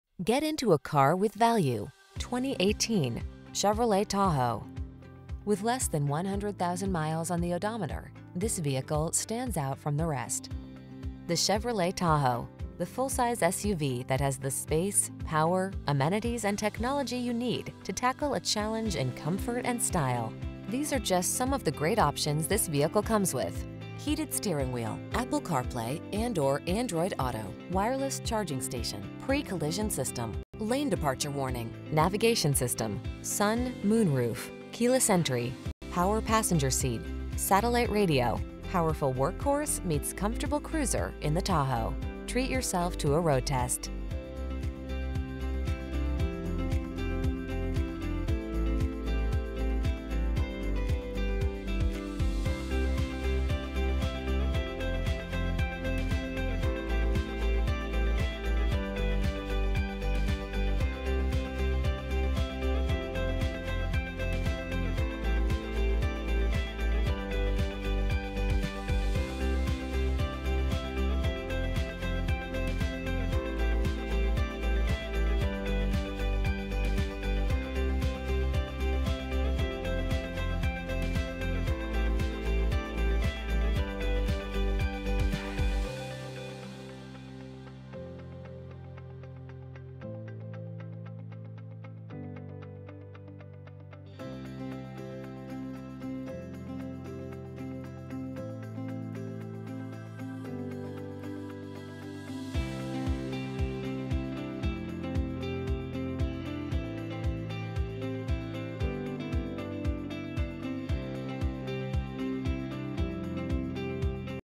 2018 Chevrolet Tahoe Premier TX Ennis, Palmer, Garrett, Ensign, Rice
Pre-Owned 2018 Chevrolet Tahoe Premier available at Pegasus Chevrolet located in 401 S I-45, ENNIS, TX, 75119. Servicing the Ennis, Palmer, Garrett, Ensign, Rice area.

Get into a car with value, the   2018  Chevrolet Tahoe.  The Chevrolet Tahoe: The full-size SUV that has the space, power, amenities, and technology you need to tackle a challenge in comfort and style.  These are just some of the great options this vehicle comes with: Heated Steering Wheel
, Apple Carplay®/Android Auto®, Wireless Charging Station, Pre-Collision System, Lane Departure Warning, Navigation System, Sun/Moonroof, Keyless Entry, Power Passenger Seat, Satellite Radio. Powerful workhorse meets comfortable cruiser in the Tahoe. Treat yourself to a road test.